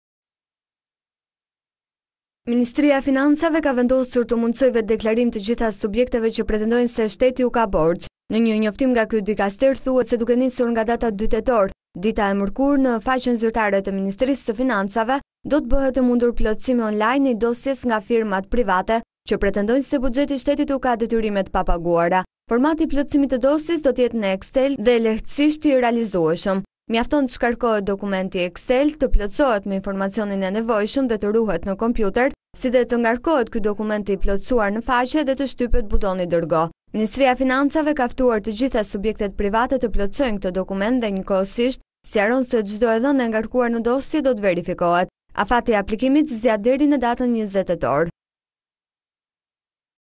Risia, ministria e Financave hap "dritare" për borxhin ndaj privatëve
TIRANE-  Ministria e Financave ka vendosur t'u mundësojë vetëdeklarimin të gjithë subjekteve që pretendojnë se shteti u ka borxh. Në një njoftim nga ky dikaster, thuhet se duke nisur nga data 2 tetor, dita e mërkurë, në faqen zyrtare të Ministrisë së Financave, do të bëhet e mundur plotësimi on-line i dosjes nga firmat private që pretendojnë se buxheti i shtetit u ka detyrime të papaguara.

Formati i plotësimit të dosjes do të jetë në exel dhe shumë lehtë i realizueshëm. Mjafton të ndiqen veprimet si më poshtë:

1. Të shkarkohet dokumenti Excel, i cili ndodhet online.

2. Të plotësohet me informacionin e nevojshëm dhe të ruhet në kompjuter.

3. Të ngarkohet dokumenti excel i plotësuar, në faqe dhe të shtypet butoni "Dërgo".

Ministria e Financave fton të gjitha subjektet private të plotësojnë dokumentin dhe njëkohësisht sqaron se çdo e dhënë e ngarkuar në dosje do të verifikohet. Afati i aplikimit zgjat deri më datë 20 tetor.